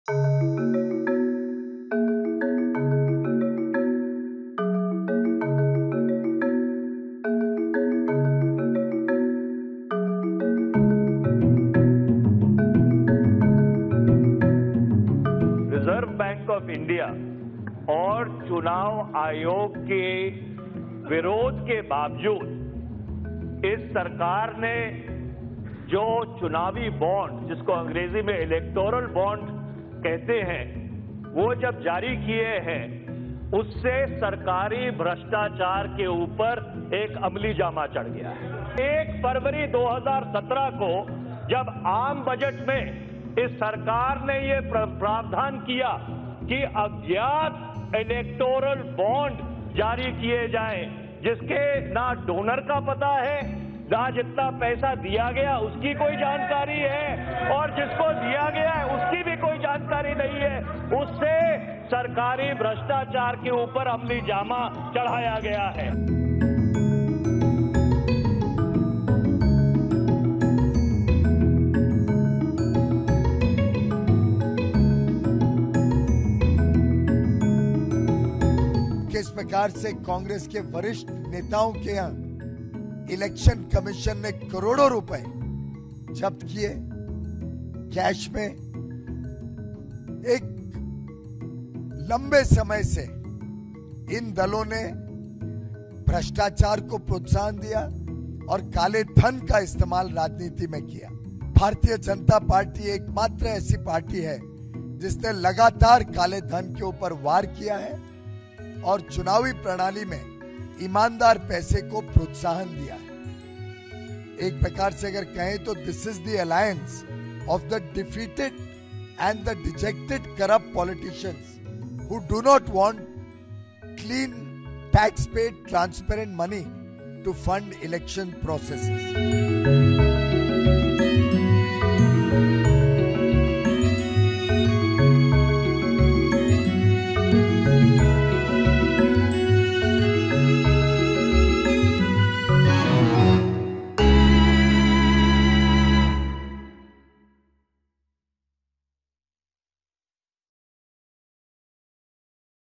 Congress Terms Electoral Bonds ‘Threat To Democracy’, Demands JPC Probe
Stepping up its attack on the Centre over the electoral bonds issue, the Congress sought an inquiry by a joint parliamentary committee into these "opaque" donations.